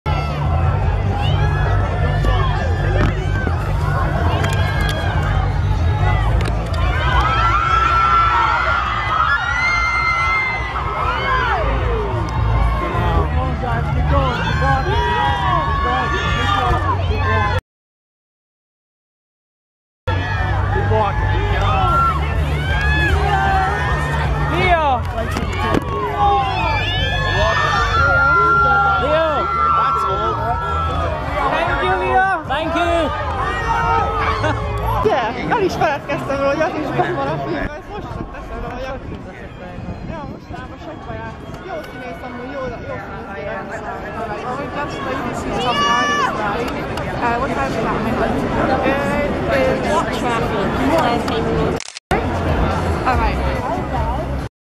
Leonardo DiCaprio's Tongue at Revenant Premiere
Leonardo DiCaprio Signing At London Premiere Excitedly
Images of me at Premiere